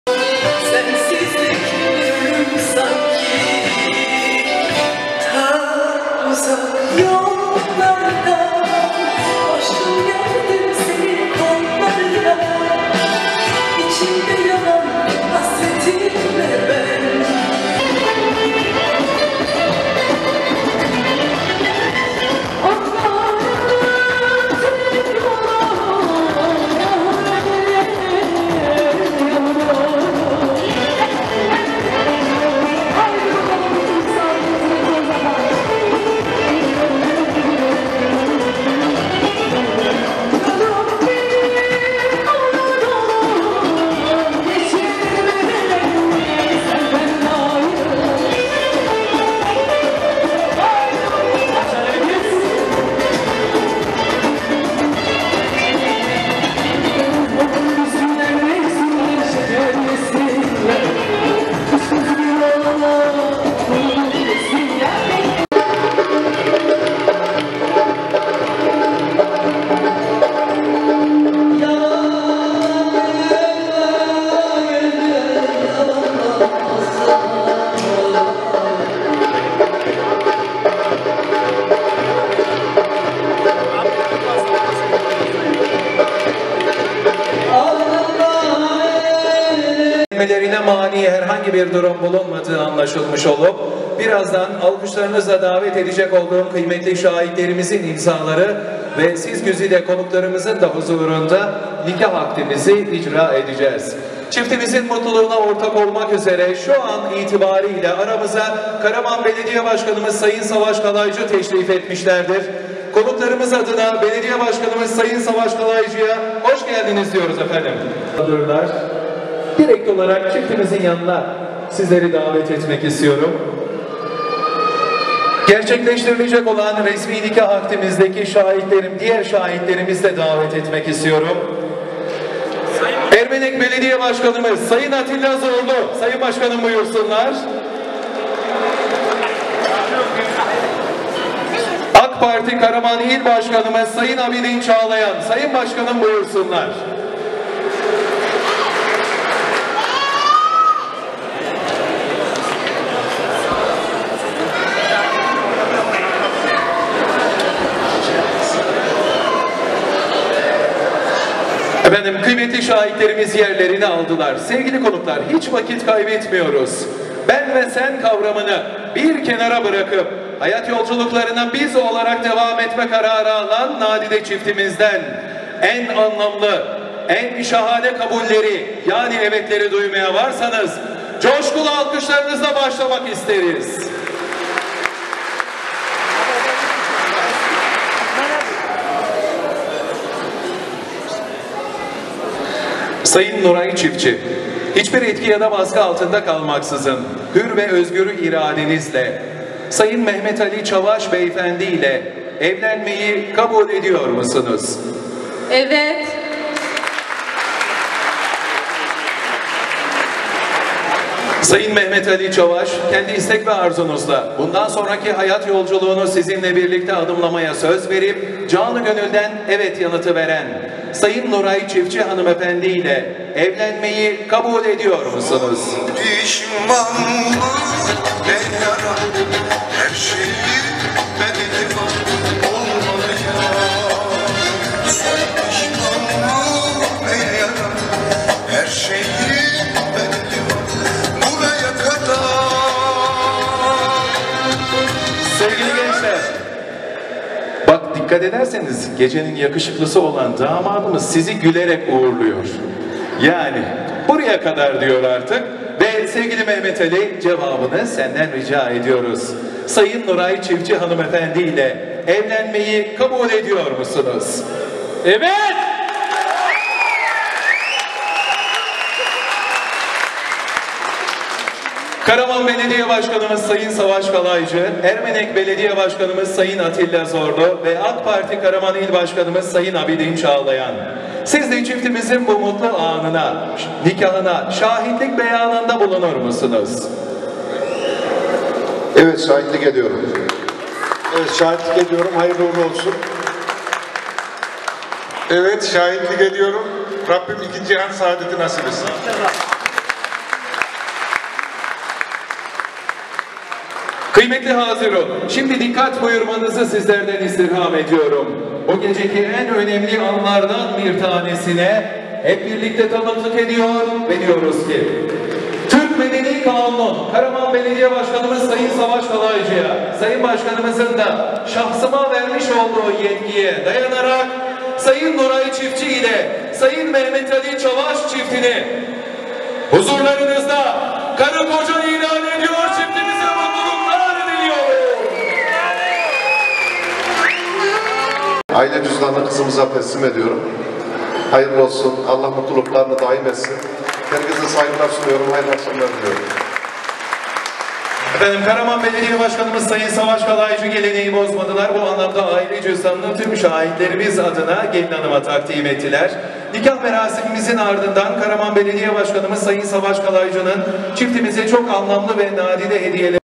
Çavaş ve Çiftçi Ailelerinin Mutlu Günü
Karaman’ın genç iş insanlarından Mehmet Ali Çavaş, Nuray Çiftçi ile mutluluğa evet dedi. - Karaman'dan Güncel Haberler